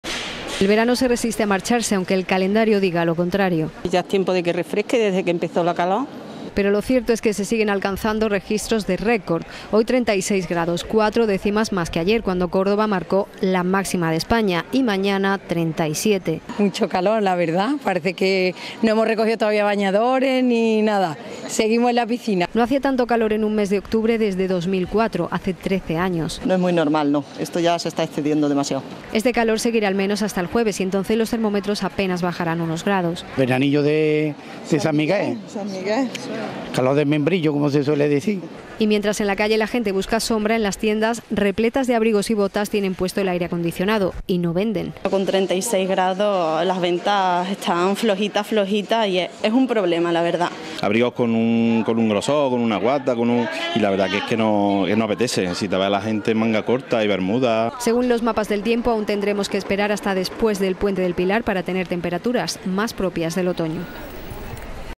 Calor
Córdoba ha iniciado el otoño con un calor impropio de estas fechas. La capital marcó ayer la máxima de todo el país, con 35'6 grados, y hoy va camino de alcanzar otro record. Los comerciantes se lamentan: con estos registros, no venden.